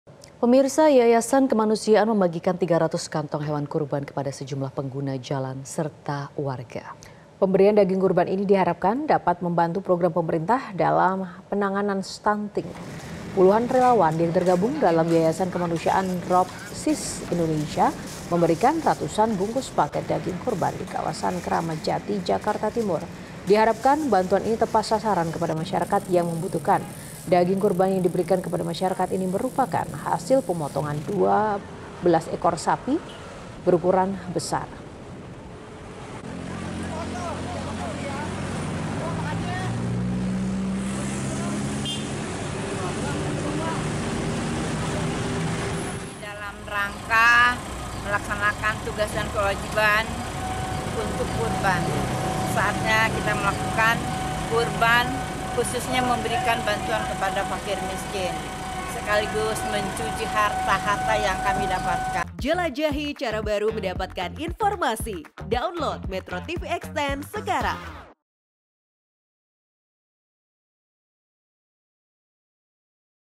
300 Kantong Daging Kurban Dibagikan ke Jalan
MetroTV, Yayasan kemanusiaan membagikan 300 kantong hewan kurban kepada sejumlah pengguna jalan serta warga. Pemberian Daging Kurban ini diharapkan dapat membantu program pemerintah dalam penanganan stunting.